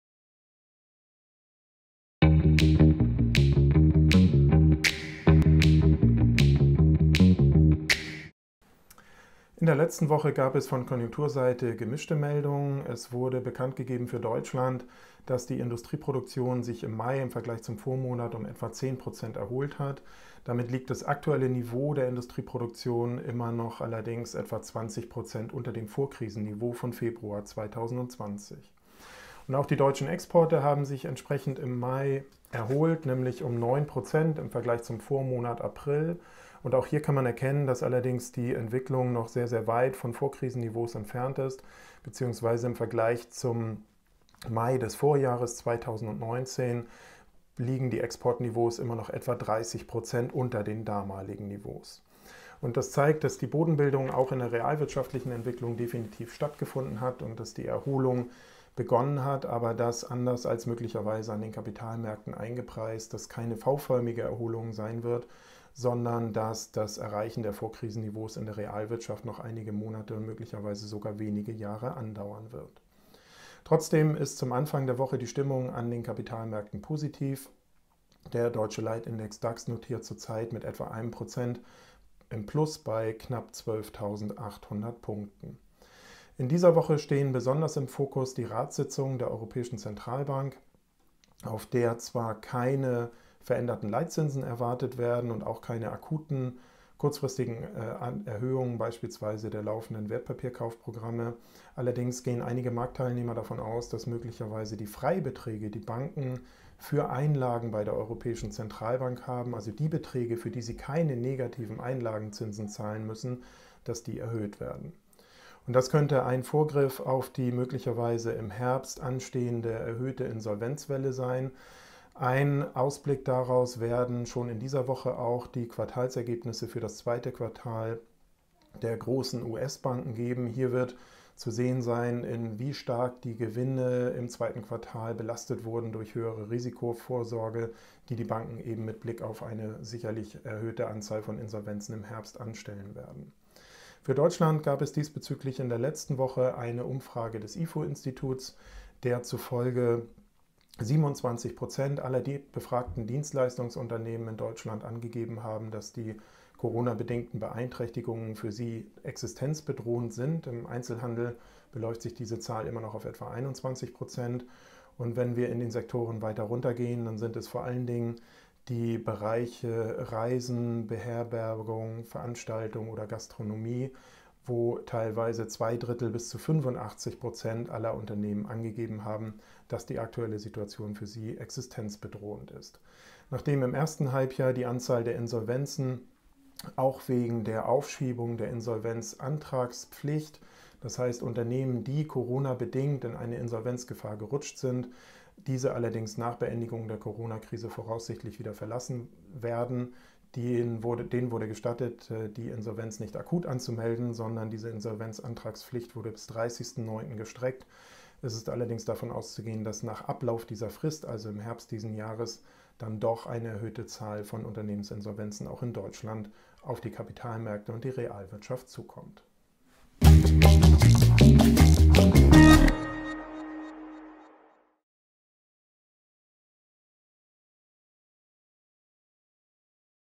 2020/07/13 - DONNER & REUSCHEL Chefvolkswirt Carsten Mumm analysiert
Carsten Mumm, Chefvolkswirt des Bankhauses DONNER & REUSCHEL, ordnet während der Corona-Krise die aktuellen Ereignisse für Sie ein.
Die aktuellen Entwicklungen der deutschen Industrieproduktion und der Exporte im Mai verdeutlichen, dass sich die Realwirtschaft wieder auf dem Wachstumspfad befindet. Allerdings wird es bis zur Erreichung der Vorkrisenniveaus noch einige Monate dauern. In dieser Woche steht die Ratssitzung der Europäischen Zentralbank im Blick, auf der eine Anhebung der negativzinsfreien Freibeträge für Einlagen der Banken bei der EZB beschlossen werden könnte. Dies könnte als Vorgriff auf die ab kommenden Herbst zu erwartende steigende Anzahl von Unternehmensinsolvenzen erfolgen.

WEITERE INFORMATIVE VIDEOS VON DONNER & REUSCHEL

FOLGEN SIE UNSEREN SOCIAL-MEDIA-KANÄLEN